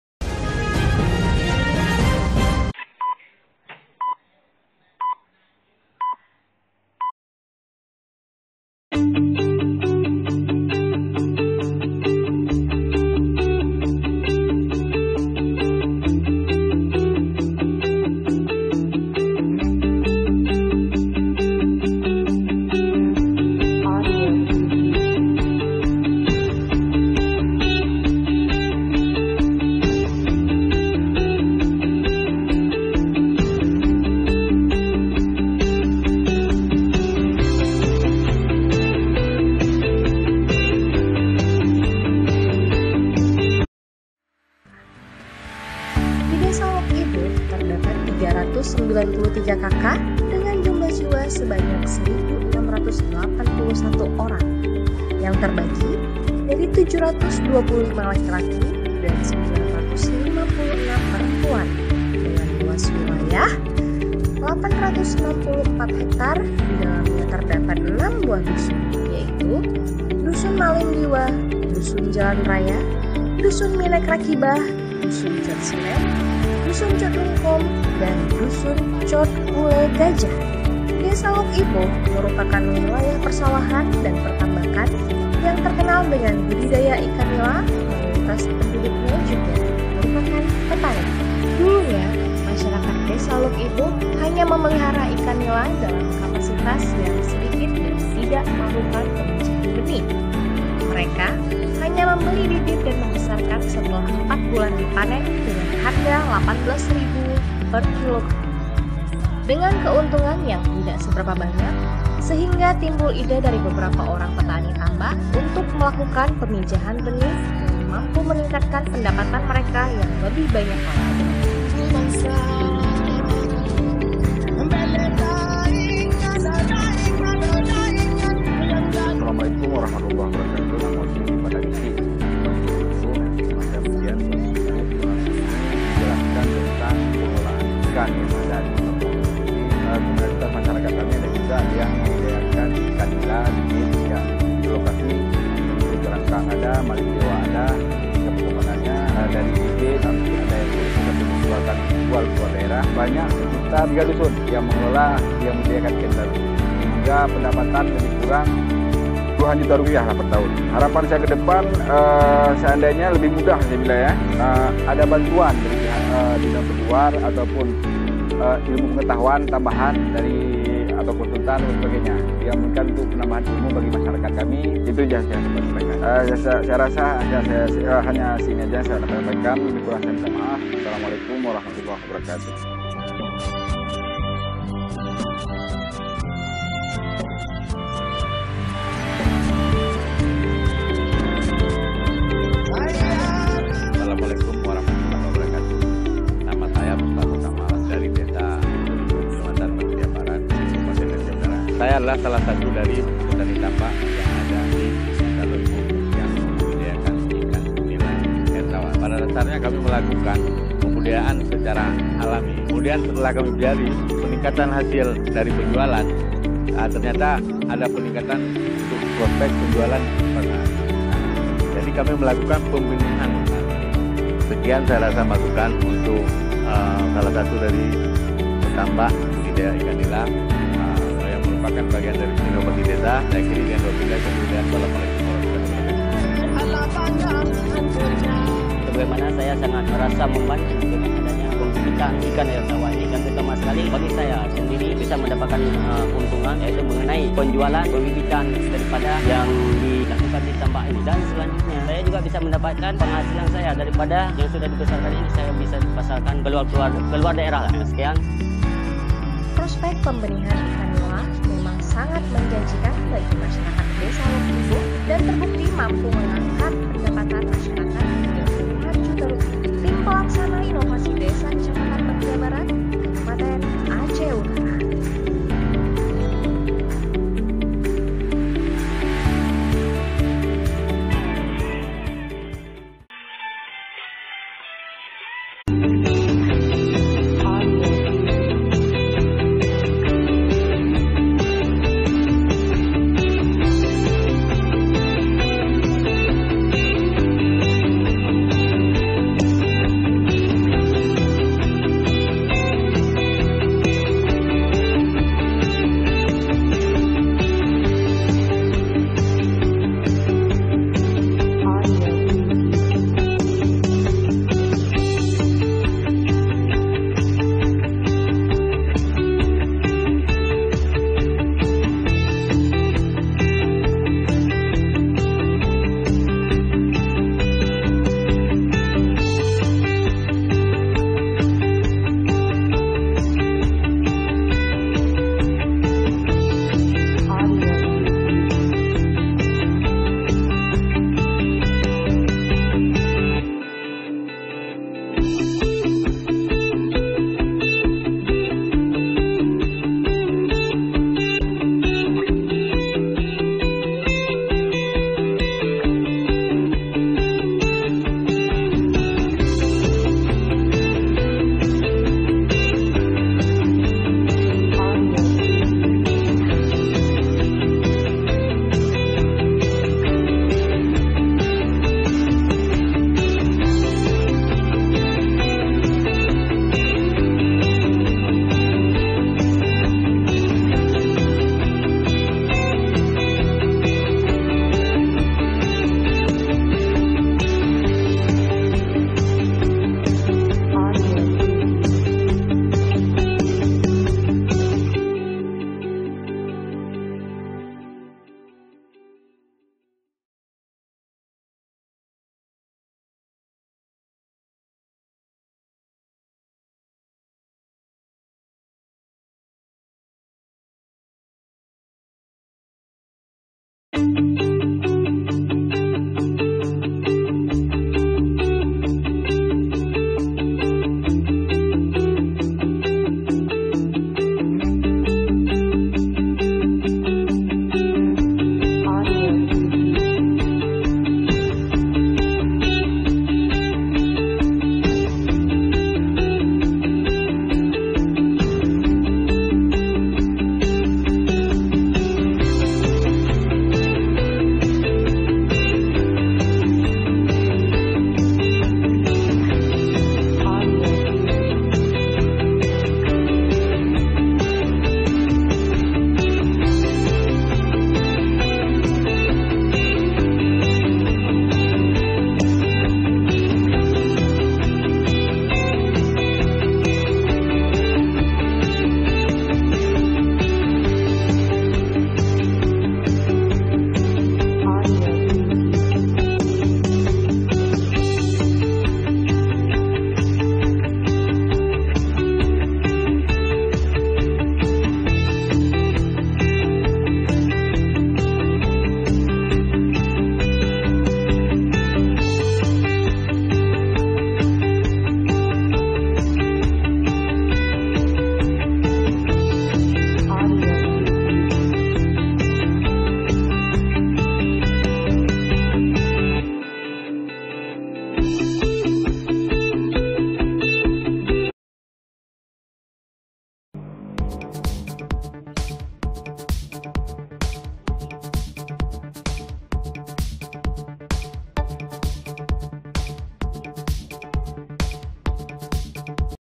BUDIDAYA IKAN NILA UNTUNG BESAR
Budidaya Ikan Nila yang Mendatangkan Keuntungan Besar untuk memotivasi semangat kita
Kenapa mereka sukses
Bagaimana trik sukses yang harus ada pada diri kita
Semoga bermanfaat